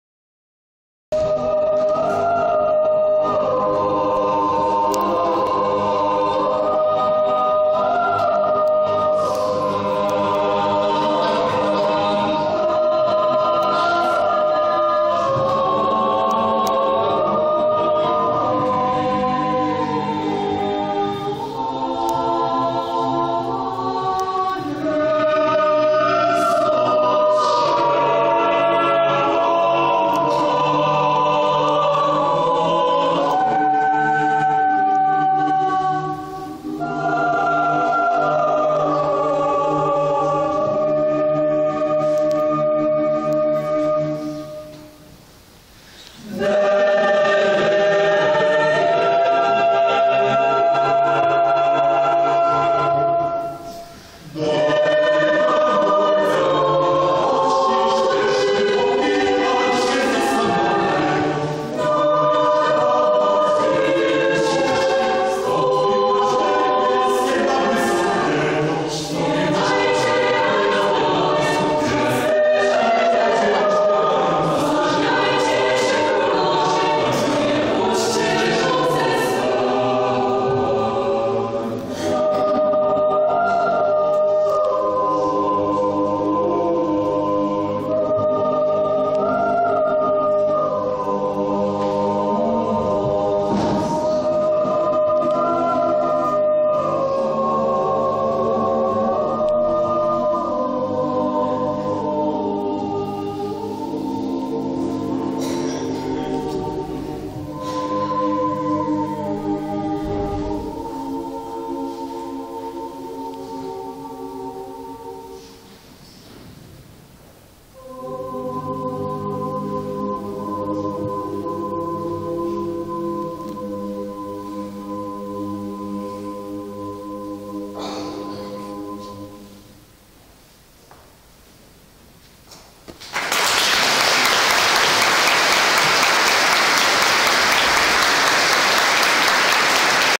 Krapkowicka Niedziela Kolędowa - Kościół pw. WNMP w Krapkowicach 29.01.2012
Koncert "Krapkowicka Niedziela Kolędowa" - 29 stycznia 2012,
(Uczta muzyczna dla serca i ucha),
Występuje: Jasnogórski Zespół Wokalny CAMRATA
Miejsce: Kościół pw. Wniebowzięcia Najświętszej Maryi Panny w Krapkowicach - Otmęcie
Organizator: Parafia pw. WNMP w Krapkowicach - Otmęcie oraz Towarzystwo Społeczno - Kulturalne Niemców na Śląsku Opolskiem - Koło Otmęt,